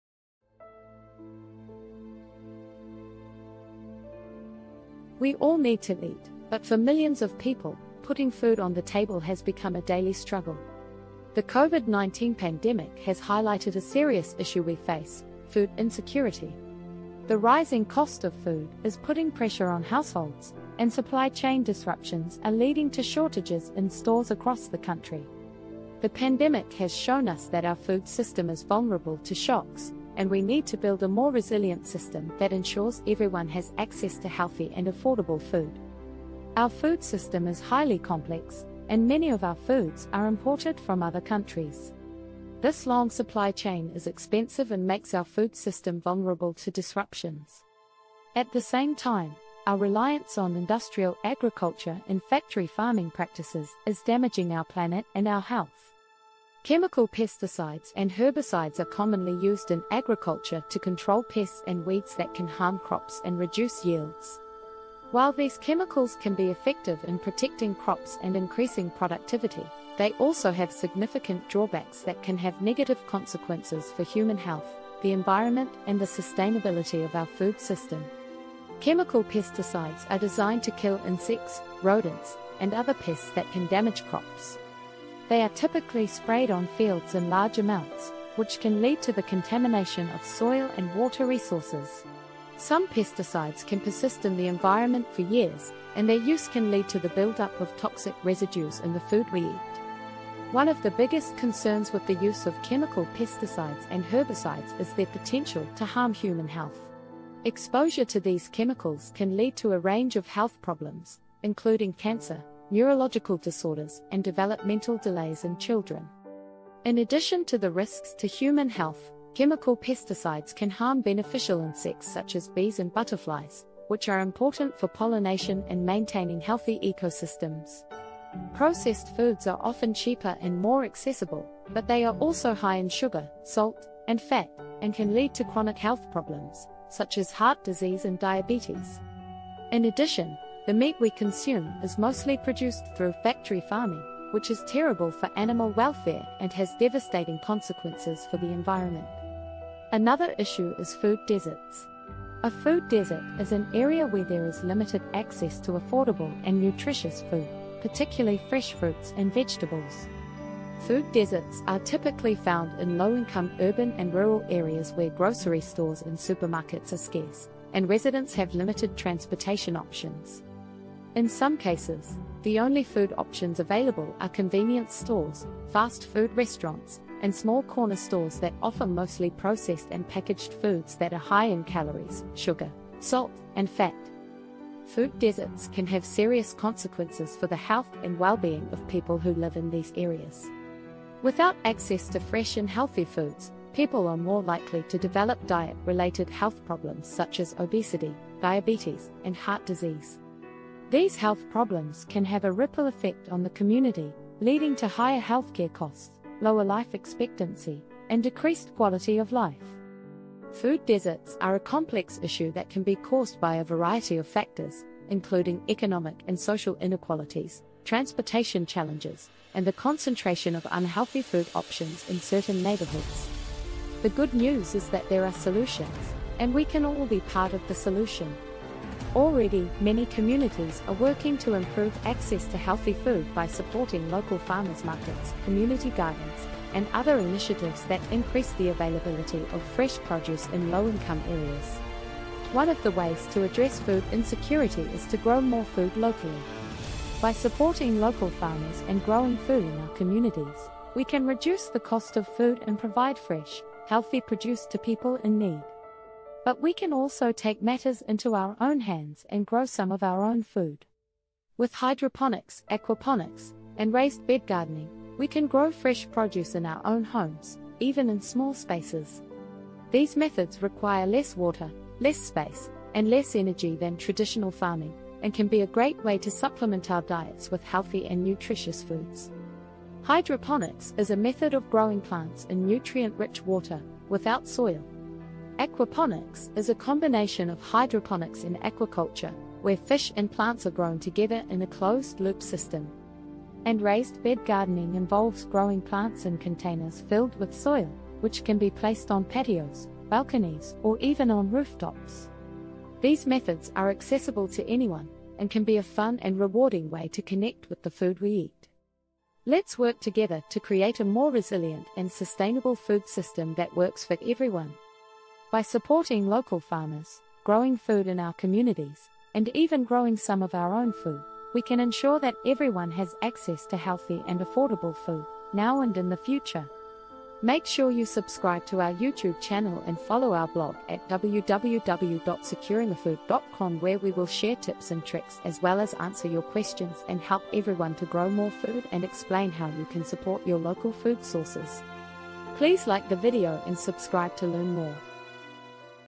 Can We Fix Our Food System?
This video delves into the complex issue of food insecurity, discussing the various causes of the problem, such as poverty, economic inequality, the use of chemical pesticides and herbicides in agriculture, and the environmental impact of industrial agriculture and factory farming practices. The video also highlights the dangers of processed foods and the importance of promoting sustainable and environmentally friendly farming practices to ensure access to healthy, nutritious food for all.